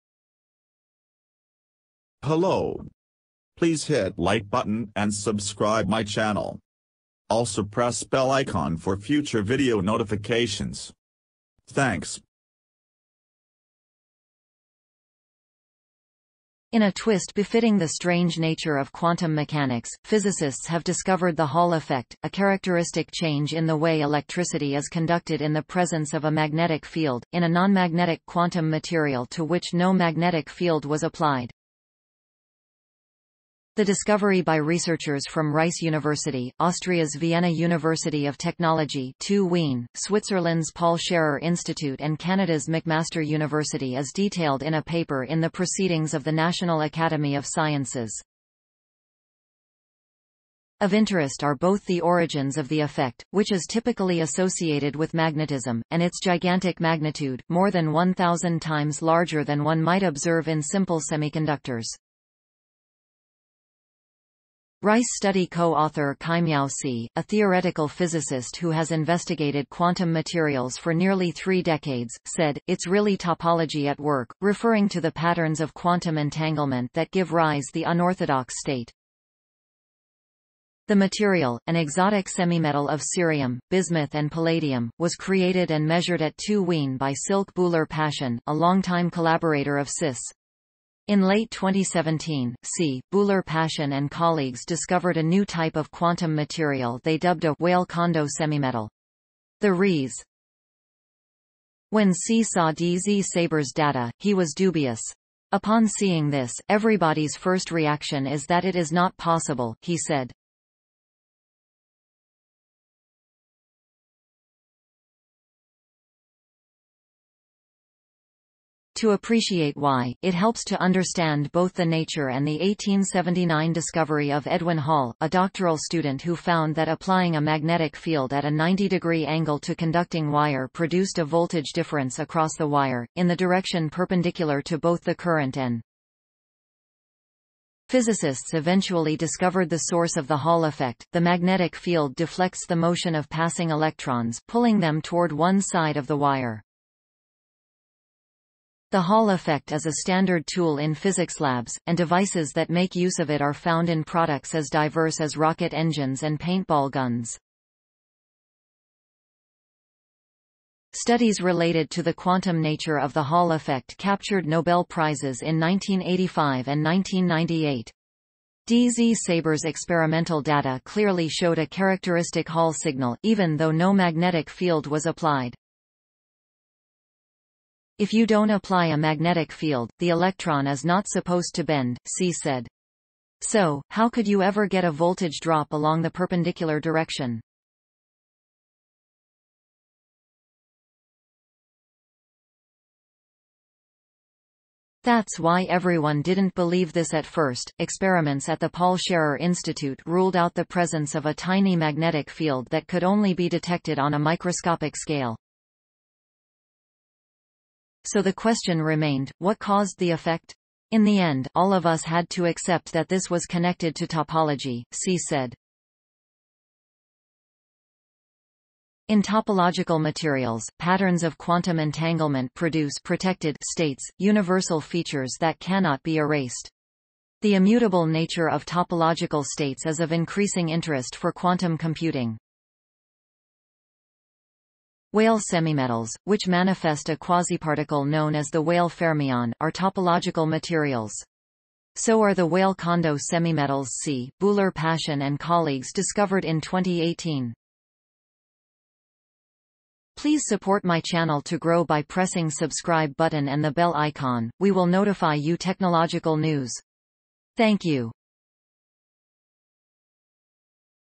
Quantum quirk yields giant magnetic effect, where none should exist
Quantum quirk yields giant magnetic effect, where none should exist - Information for all latest updates Science and Technology

In a twist befitting the strange nature of quantum mechanics, physicists have discovered the Hall effect -- a characteristic change in the way electricity is conducted in the presence of a magnetic field -- in a nonmagnetic quantum material to which no magnetic field was applied. The discovery by researchers from Rice University, Austria's Vienna University of Technology (TU Wien), Switzerland's Paul Scherrer Institute and Canada's McMaster University is detailed in a paper in the Proceedings of the National Academy of Sciences. Of interest are both the origins of the effect, which is typically associated with magnetism, and its gigantic magnitude -- more than 1,000 times larger than one might observe in simple semiconductors.
Rice study co-author Qimiao Si, a theoretical physicist who has investigated quantum materials for nearly three decades, said, "It's really topology at work," referring to the patterns of quantum entanglement that give rise the unorthodox state.
The material, an exotic semimetal of cerium, bismuth and palladium, was created and measured at TU Wien by Silke Bühler-Paschen, a longtime collaborator of Si's. In late 2017, Si, Bühler-Paschen and colleagues discovered a new type of quantum material they dubbed a "Weyl-Kondo semimetal." The research laid the groundwork for empirical investigations, but Si said the experiments were challenging, in part because it wasn't clear "which physical quantity would pick up the effect."
In April 2018, Bühler-Paschen and TU Wien graduate student Sami Dzsaber, the study's first author, dropped by Si's office while attending a workshop at the Rice Center for Quantum Materials (RCQM). When Si saw Dzsaber's data, he was dubious.
"Upon seeing this, everybody's first reaction is that it is not possible," he said. To appreciate why, it helps to understand both the nature and the 1879 discovery of Edwin Hall, a doctoral student who found that applying a magnetic field at a 90-degree angle to conducting wire produced a voltage difference across the wire, in the direction perpendicular to both the current and the magnetic field. Physicists eventually discovered the source of the Hall effect: The magnetic field deflects the motion of passing electrons, pulling them toward one side of the wire. The Hall effect is a standard tool in physics labs, and devices that make use of it are found in products as diverse as rocket engines and paintball guns. Studies related to the quantum nature of the Hall effect captured Nobel Prizes in 1985 and 1998.
Dzsaber's experimental data clearly showed a characteristic Hall signal, even though no magnetic field was applied.
"If you don't apply a magnetic field, the electron is not supposed to bend," Si said. "So, how could you ever get a voltage drop along the perpendicular direction? That's why everyone didn't believe this at first."
Experiments at the Paul Scherrer Institute ruled out the presence of a tiny magnetic field that could only be detected on a microscopic scale. So the question remained: What caused the effect?
"In the end, all of us had to accept that this was connected to topology," Si said. In topological materials, patterns of quantum entanglement produce "protected" states, universal features that cannot be erased. The immutable nature of topological states is of increasing interest for quantum computing. Weyl semimetals, which manifest a quasiparticle known as the Weyl fermion, are topological materials.
So are the Weyl-Kondo semimetals Si, Bühler-Paschen and colleagues discovered in 2018. Those feature both Weyl fermions and the Kondo effect, an interaction between the magnetic moments of electrons attached to atoms inside the metal and the spins of passing conduction electrons.
"The Kondo effect is the quintessential form of strong correlations in quantum materials," Si said in reference to the correlated, collective behavior of billions upon billions of quantum entangled particles. "It qualifies the Weyl-Kondo semimetal as one of the rare examples of a topological state that's drive...